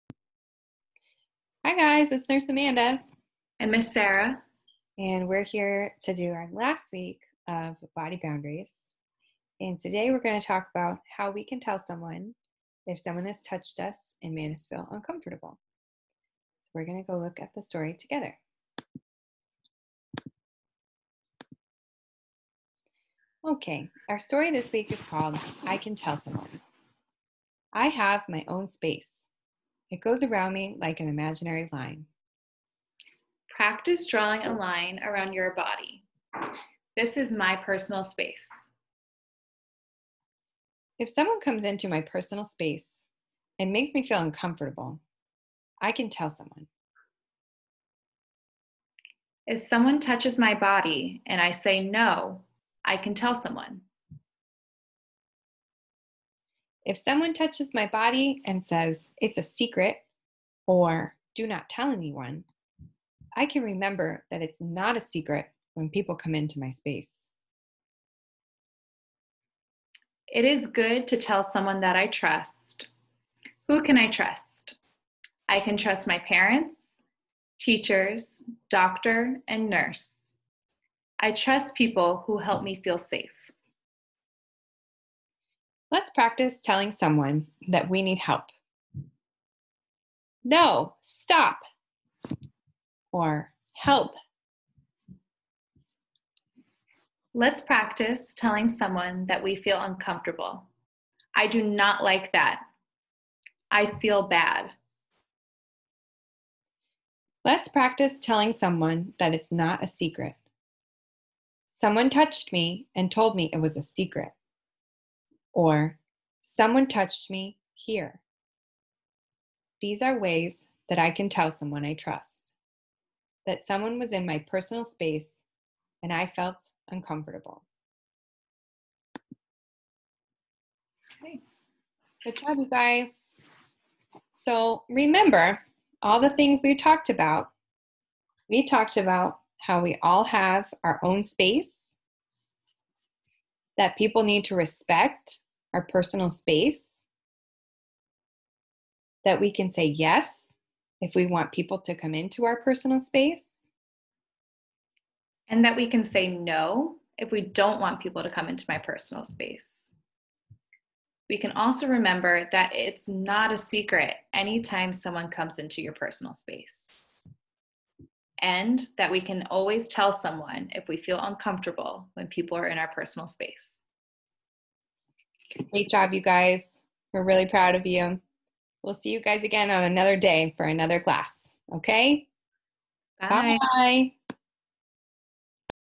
I Can Tell Someone
Nurse Amanda and Miss Sarah talk about how to tell someone if someone has come into your personal space and made you feel uncomfortable.